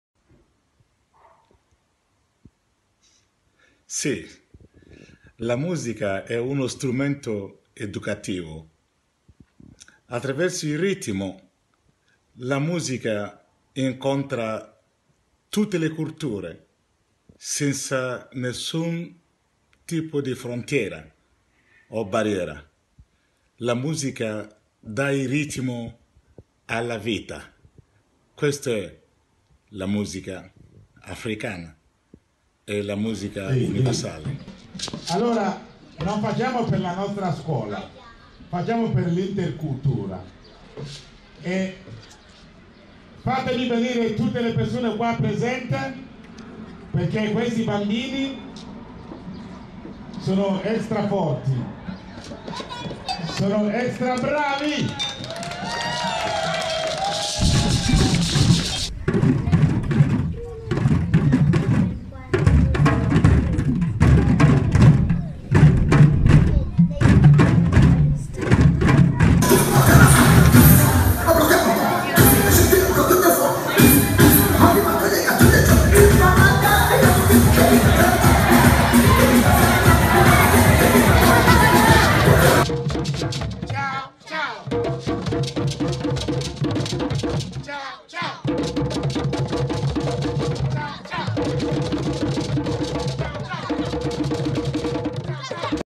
Steve Emejuru - la musica lingua universale
La musica è un linguaggio universale e camminando attraverso il mondo si trasforma, crea incontro, scambio, è uno strumento educativo molto potente: ogni bambino (ma anche adulto) si sente coinvolto e sperimenta nuovi linguaggi, le diversità culturali si amalgamano e danno vita ad espressioni nuove. Così dall'Africa insieme agli schiavi sono partite musiche che oltreoceano hanno trovato altre strade trasformandosi … soul, jazz, rap, hip hop queste sono le nuove espressioni di una musica che viene da lontano. Steve Emejuru, musicista e mediatore interculturale con poche immagini ci trasmette questa bellezza.